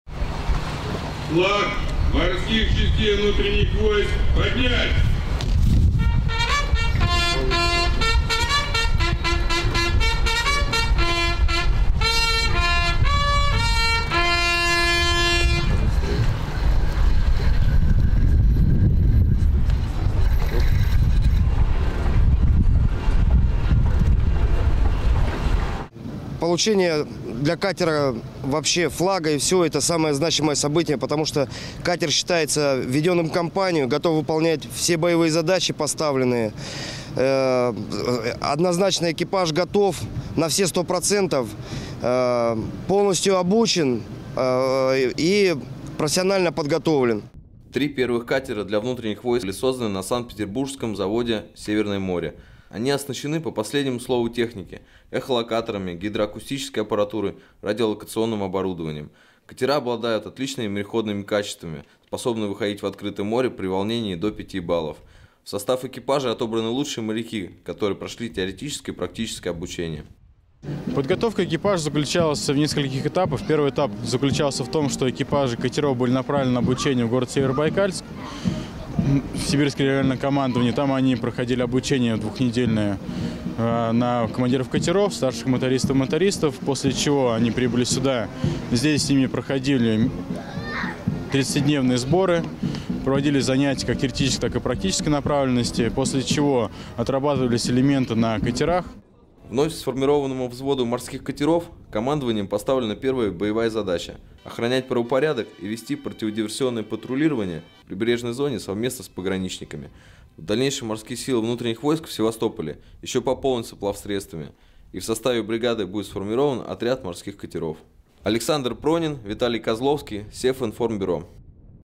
21.11.2015 Морские части Внутренних войск получили новые катера
112 бригада внутренних войск МВД России пополнилась тремя новыми морскими катерами. Утром в субботу, 21 ноября, на Графской пристани в торжественной обстановке и в присутствии командования на катерах были подняты флаги Морских частей внутренних войск.